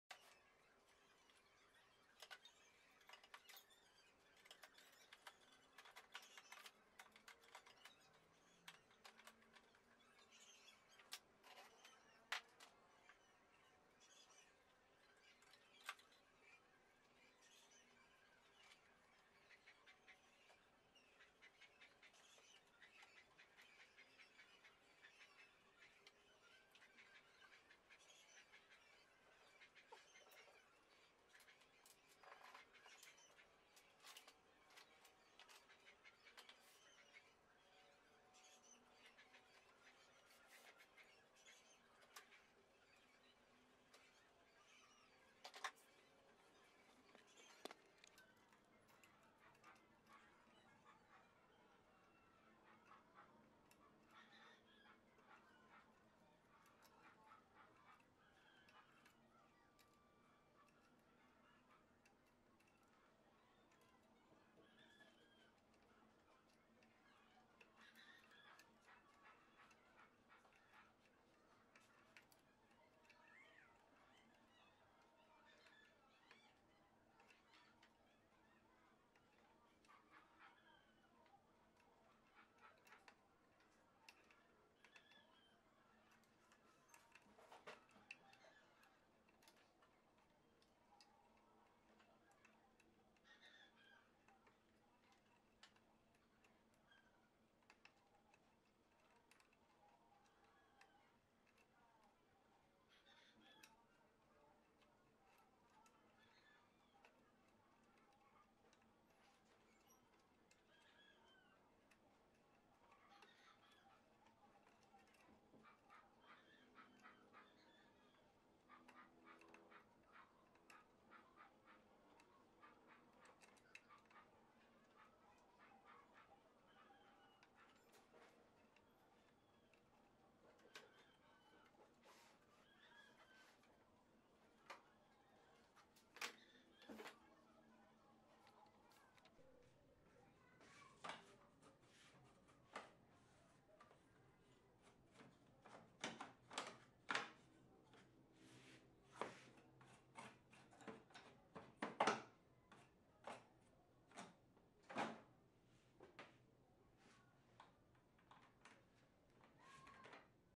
Assembling Guide - Returning of Epson T60 Motherboard
Be careful inserting the power and double check if all the data cable  is in proper position